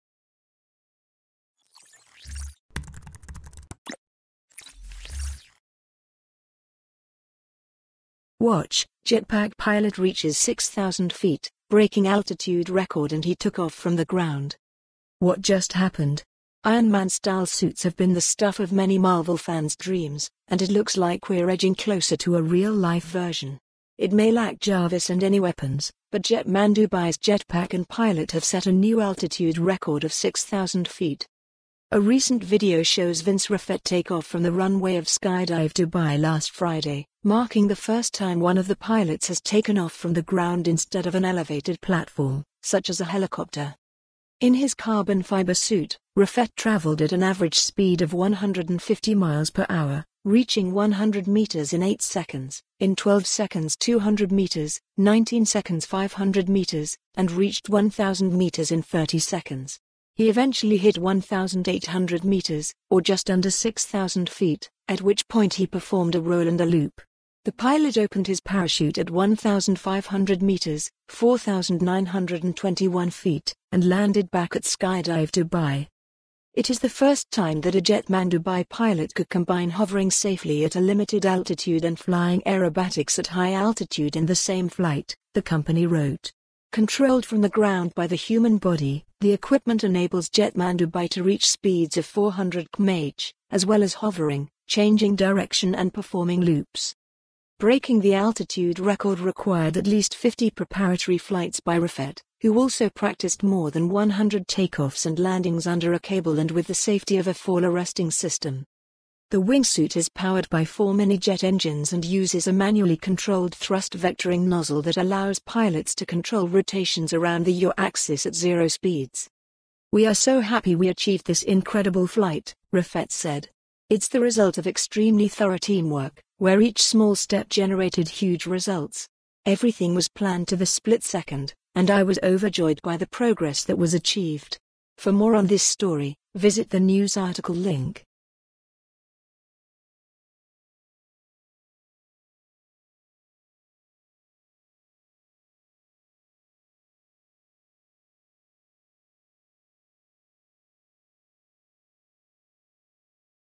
Watch: Jetpack pilot reaches 6,000 feet, breaking altitude record
And he took off from the ground

What just happened? Iron Man-style suits have been the stuff of many Marvel fans' dreams, and it looks like we're edging closer to a real-life version. It may lack Jarvis and any weapons, but Jetman Dubai's jetpack and pilot have set a new altitude record of 6,000 feet.

A recent video shows Vince Reffet take off from the runway of Skydive Dubai last Friday, marking the first time one of the pilots has taken off from the ground instead of an elevated platform, such as a helicopter.

In his carbon-fiber suit, Reffet traveled at an average speed of 150 mph, reaching 100 meters in eight seconds, "in 12 seconds 200m, 19 seconds 500m, and reached 1,000m in 30 seconds." He eventually hit 1,800 meters, or just under 6,000 feet, at which point he performed a roll and a loop. The pilot opened his parachute at 1,500 meters (4,921 feet) and landed back at Skydive Dubai.

"It is the first time that a Jetman Dubai pilot could combine hovering safely at a limited altitude and flying aerobatics at high altitude in the same flight," the company wrote. "Controlled from the ground by the human body, the equipment enables Jetman Dubai to reach speeds of 400kmh, as well as hovering, changing direction and performing loops."

Breaking the altitude record required at least 50 preparatory flights by Reffet, who also practiced more than 100 take-offs and landings under a cable and with the safety of a fall arresting system.

The wingsuit is powered by four mini jet engines and uses a manually controlled thrust vectoring nozzle that allows pilots "to control rotations around the yaw axis at zero speeds."

"We are so happy